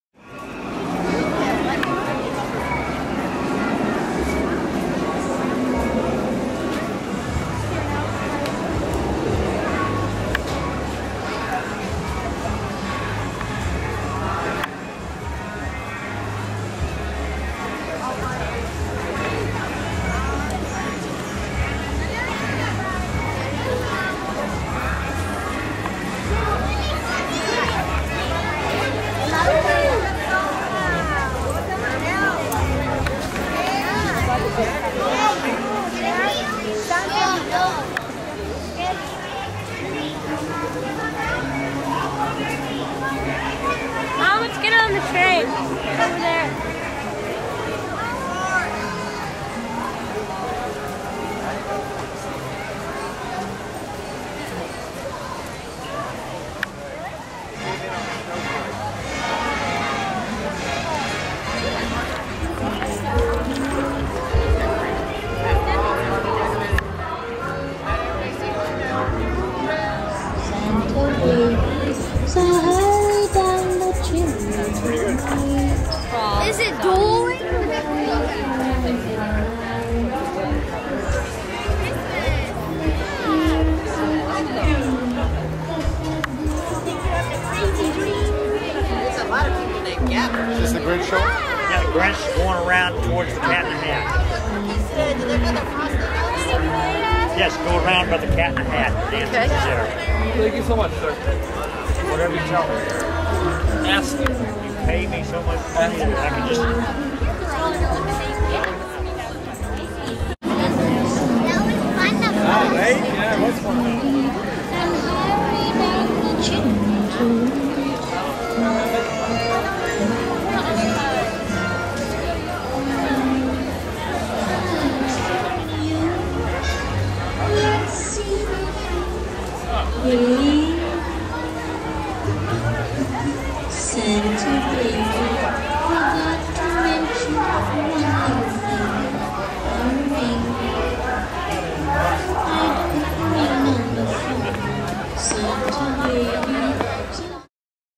The Whos of Whoville
Short video of some of the Whos of Whoville frolicking around Seuss Landing.